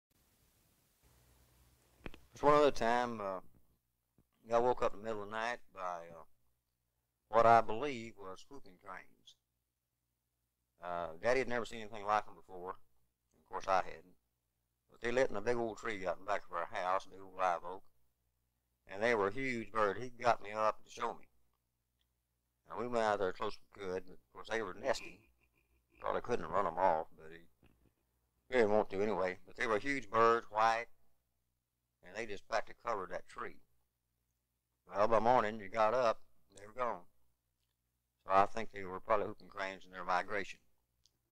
Billie Joe Goble - Whooping Crains in Mahomet
Billie Joe Goble - Whooping crains visit Mahomet Texas in the 1930s or 40's.